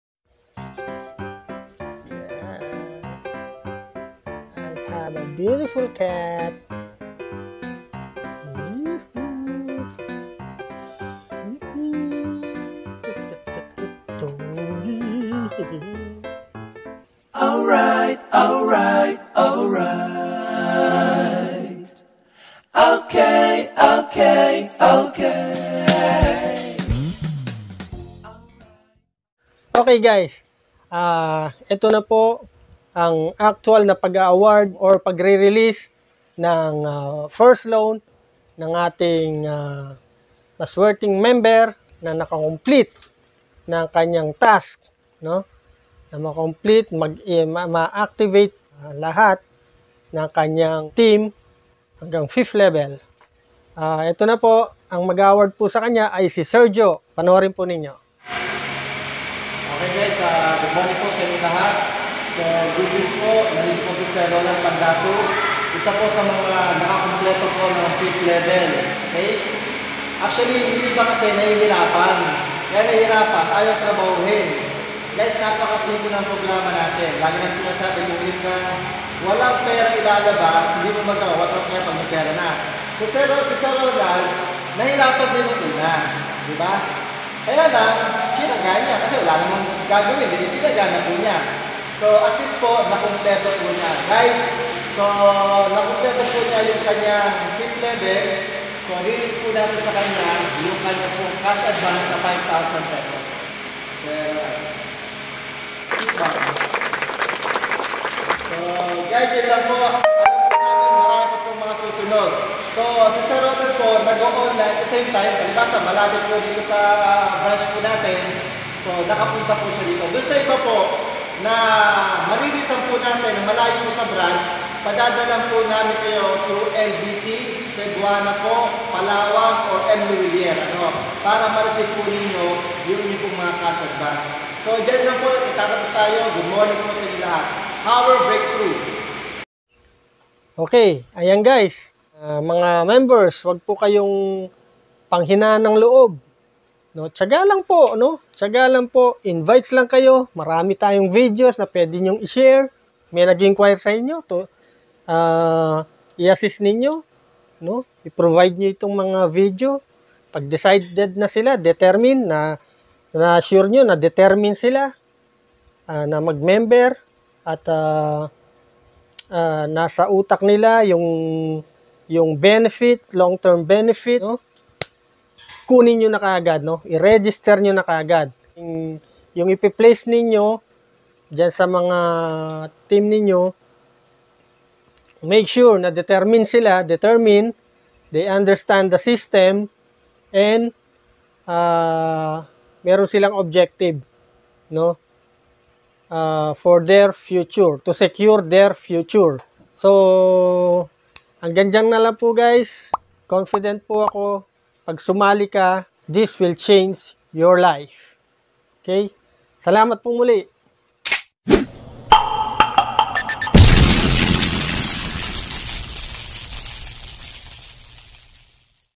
Releasing of Cash Advance | Sipag at Tiyaga
Completing his task to avail Cash Advance.